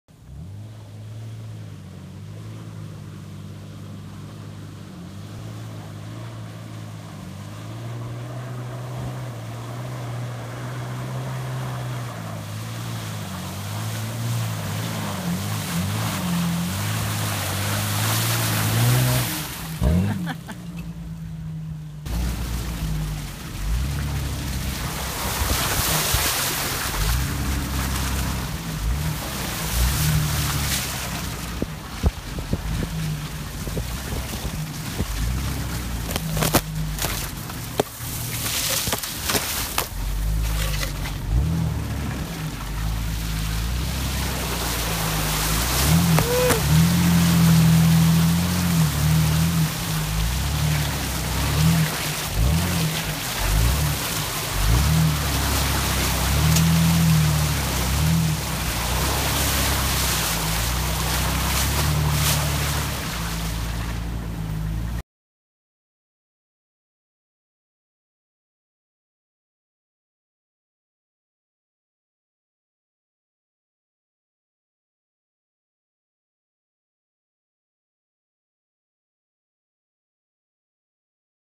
JEEP Wrangler afundando na lama - Stuck in mud
This video was made to show some trails on the J.W. Corbett Wildlife Managment Area in West Palm Beach - Florida .

Very nice place, lots of mud trails, camping area, wildlife.

Very fun place to take your jeep !!